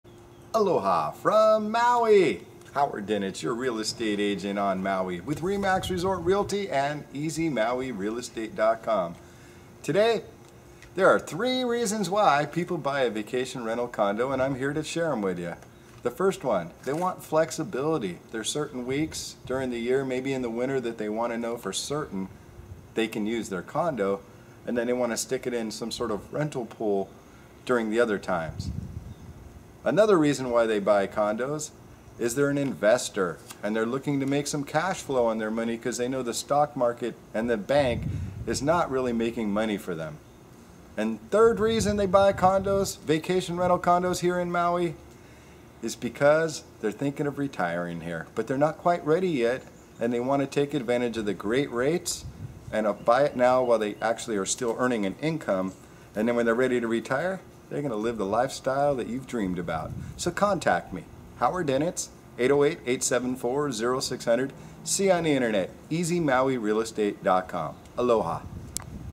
Reasons people buy a vacation rental condo in maui
Three Reasons people buy a vacation rental condo in maui.
People buy a vacation rental condo so they can block out specific dates they wish to use and put the condo in a rental pool when they are not on island. Invstors like vacation rental condos for the cash flow instead of leaving their money in the bank or the stock market, Also people thinking of retiring buy Now while they are working and can afford the purchase.